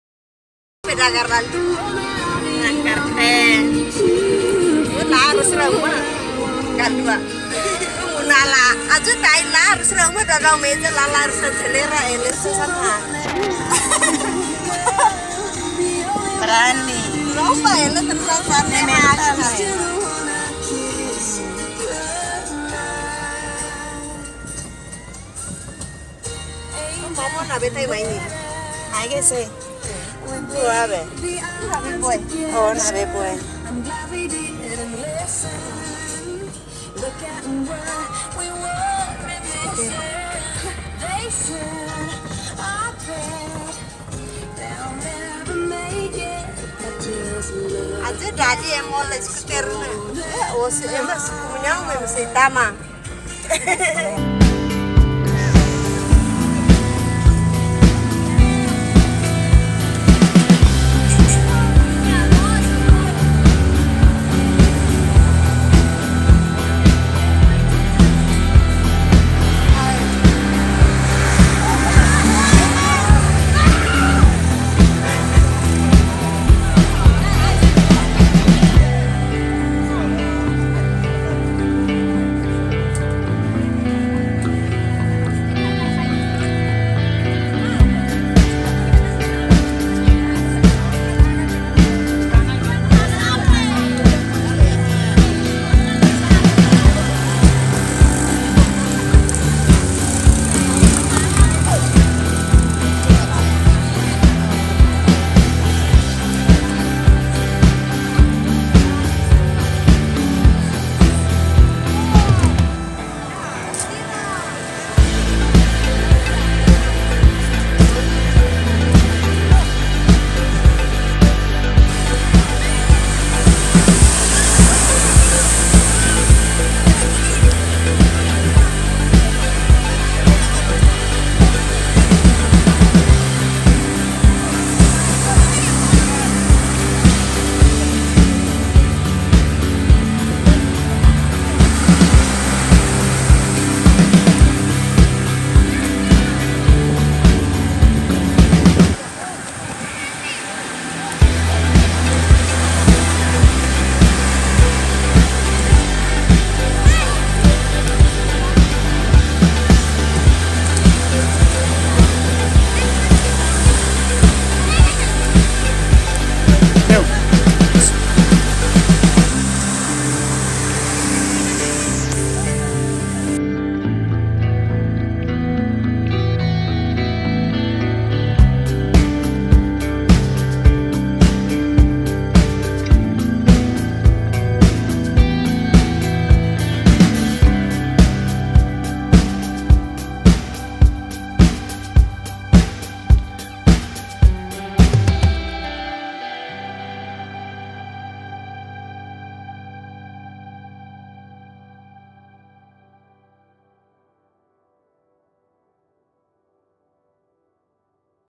MEMORIA 2014 TINAN FOUN IHA tasi lIQUISA
MEMORIA 2014 TINAN FOUN IHA lIQUISA HAMUTUK HO FAMILIA SIRA NO MALUK cAILACO SIRA IHA DELTA 3